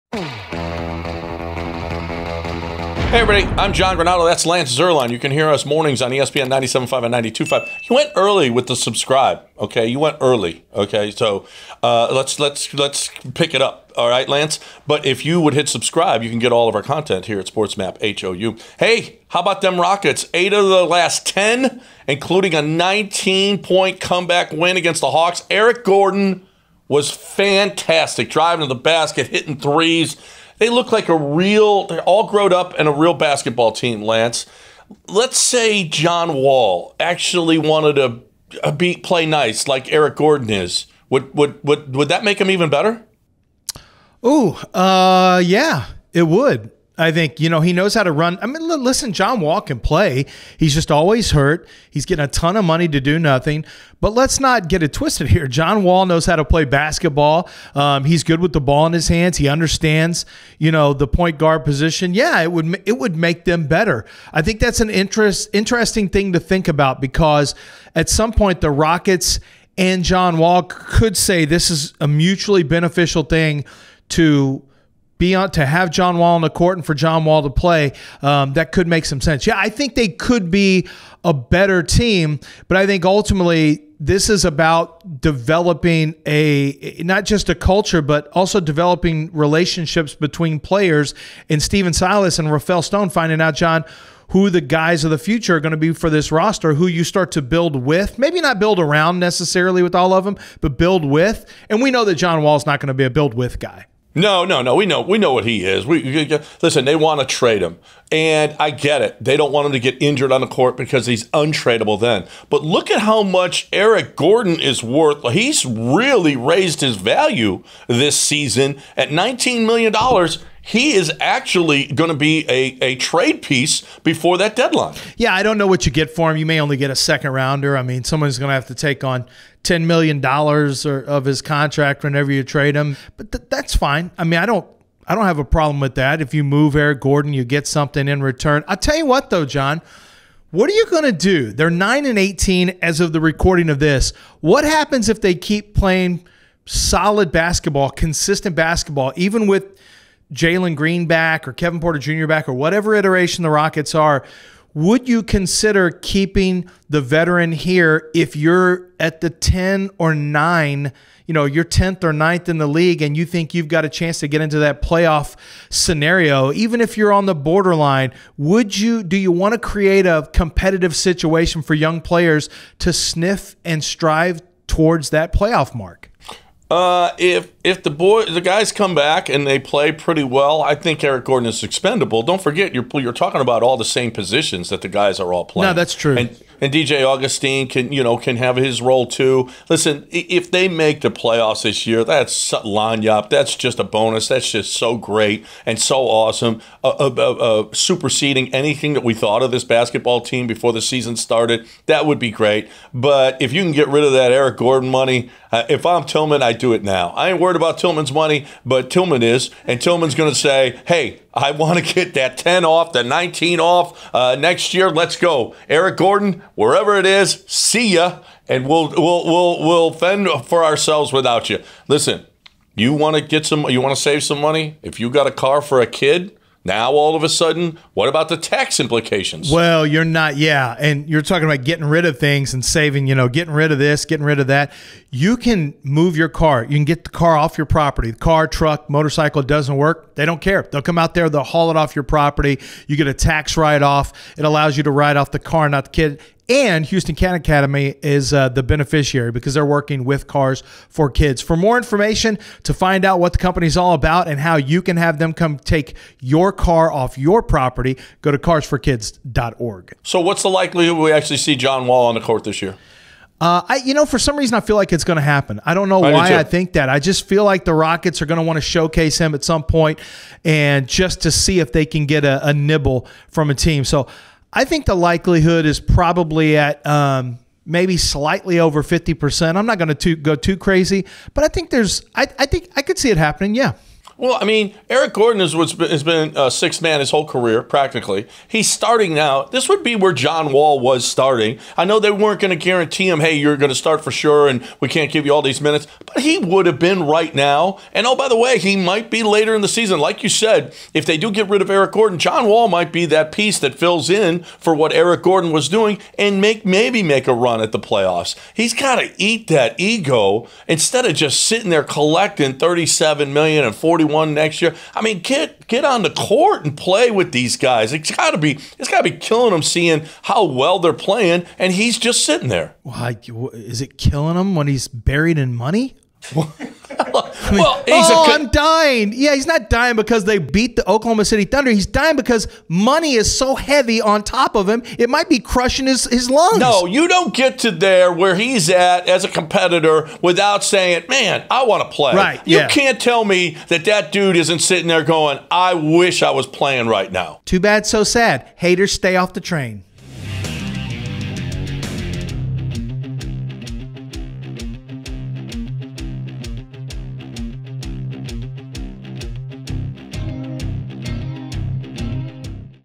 Examining the intersection of Eric Gordon’s dominance and John Wall’s future
Following an exciting comeback against the Atlanta Hawks, Houston has won 8 of their last 10 and find themselves one of the hottest teams in the NBA over that stretch. ESPN Houston's John Granato and Lance Zierlein discuss the differences in how Gordon has handled the rebuilding process versus Wall, and how Houston might navigate Wall's return.